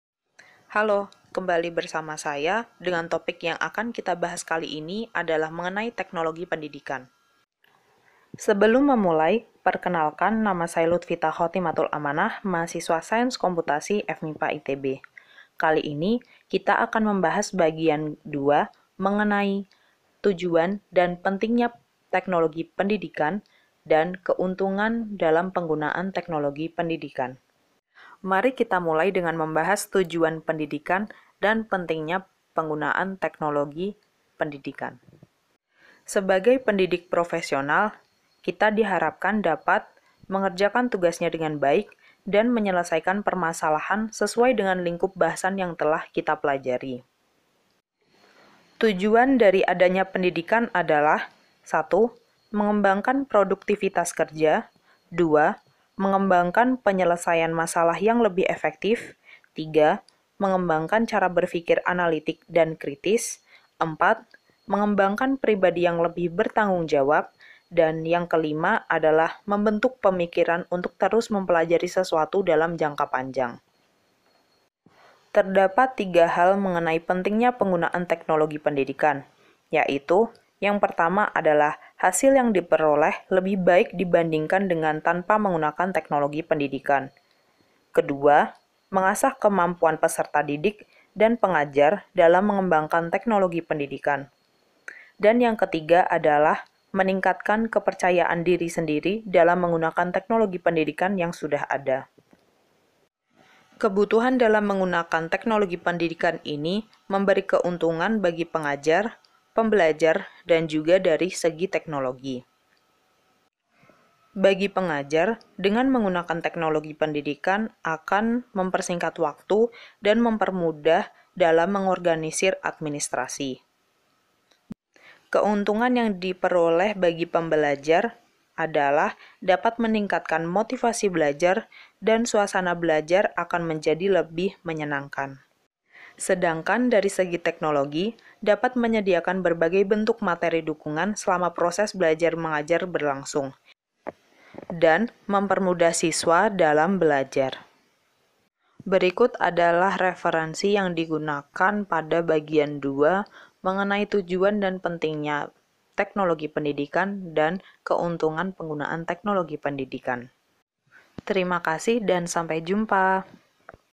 01 02 Tujuan, Pentingnya dan Keuntungan Penggunaan Teknologi Pendidikan
UAS IF6048 Pembelajaran Multimedia
Semester I - 2020/2021

Nama: Luthfita Khotimatul Amanah
NIM: 20919004

Judul Topik: Introduction of Educational Technology
Judul Segmen: Tujuan, Pentingnya dan Keuntungan Penggunaan Teknologi Pendidikan